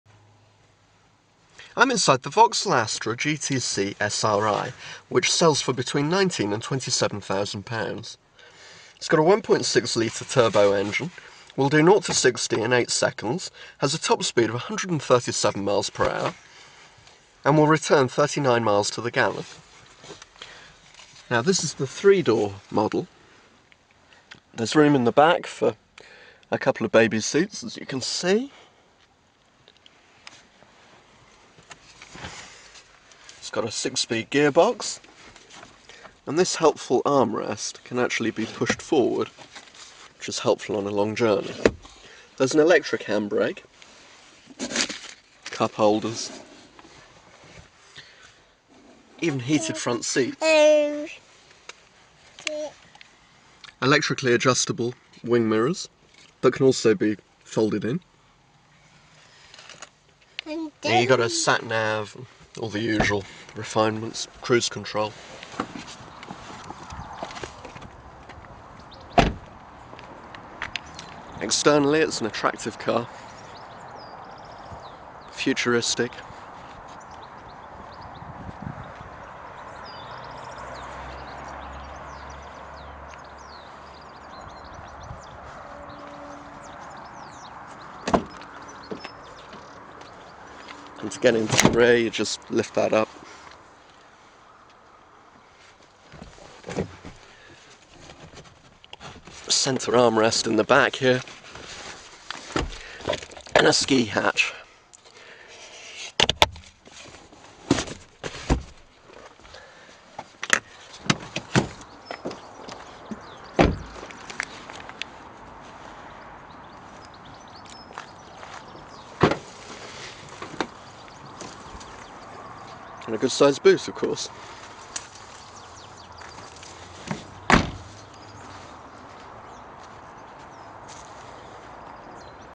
Vauxhall Astra GTC SRi
This three-door 1.6-litre petrol turbo is a sporty vehicle but large enough for the family, too.